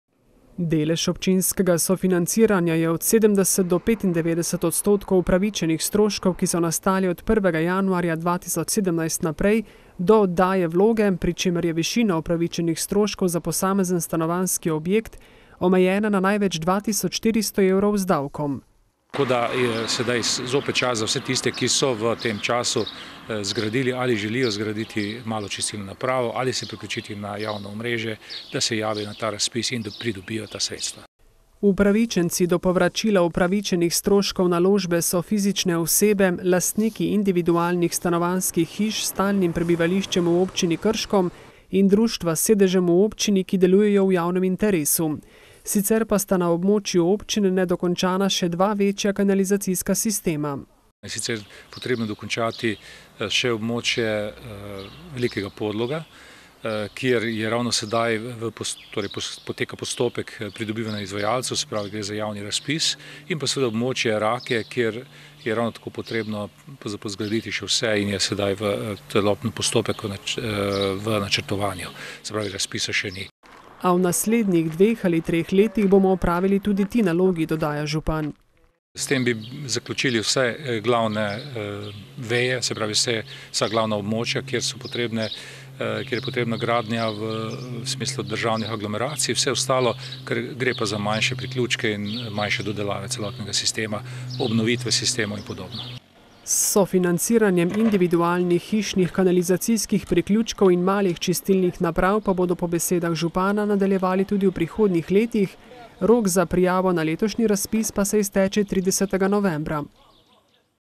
Občina sofinancira izgradnjo kanalizacije
KRŠKO - Krška občina je s ciljem urejanja kanalizacijskega omrežja tudi letos objavila javni razpis za sofinanciranje izgradnje individualnih hišnih kanalizacijskih priključkov in malih čistilnih naprav za komunalne odpadne vode.